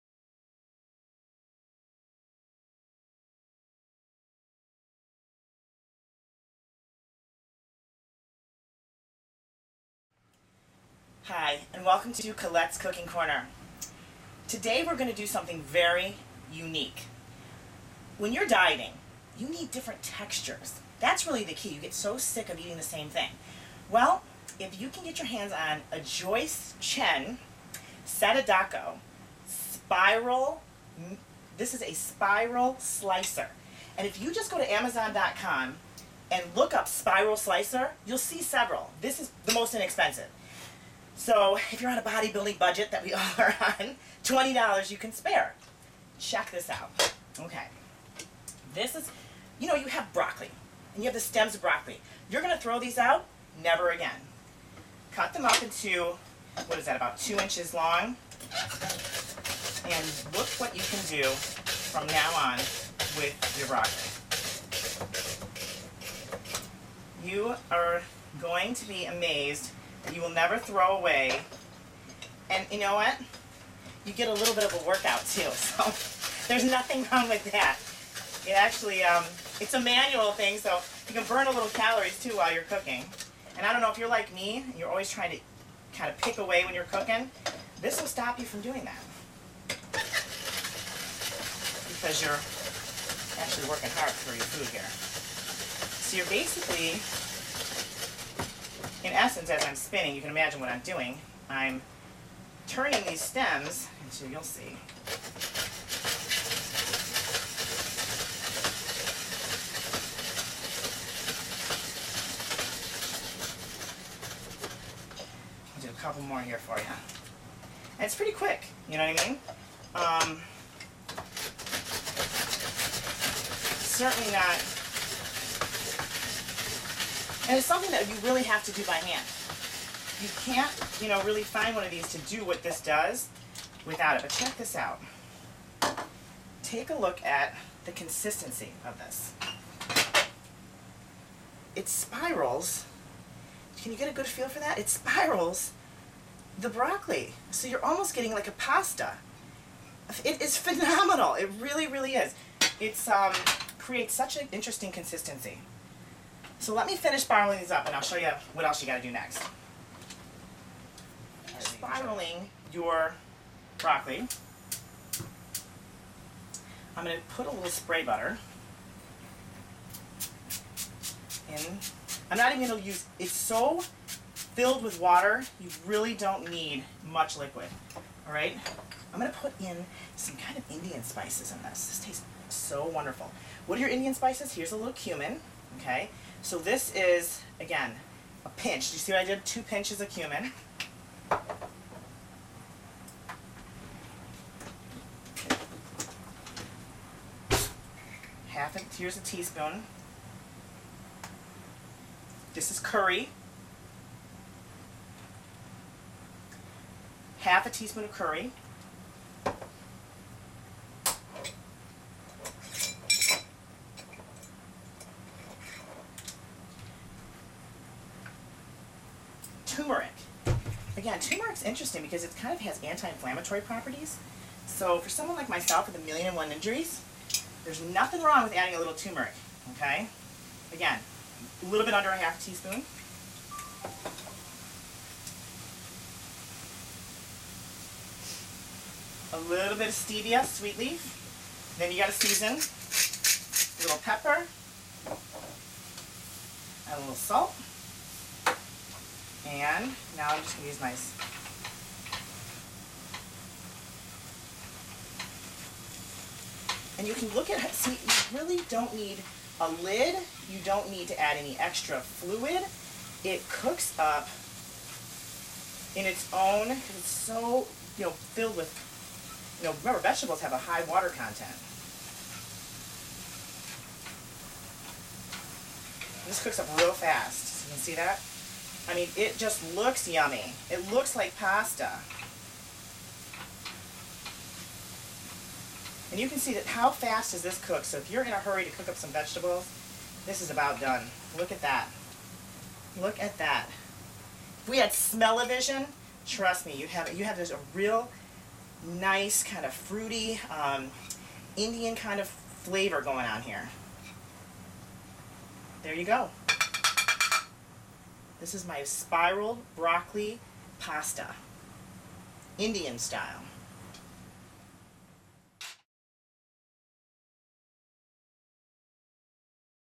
Colette's Brocolli Spiral with Appliance
Indian Style Broccoli Slaw
2 stems broccoli
1/2 tsp cumin, curry, tumeric,paprika
Spray butter
1 tbsp dijon mustard
salt/pepper

Directions: cook on high heat for 2-3 minutes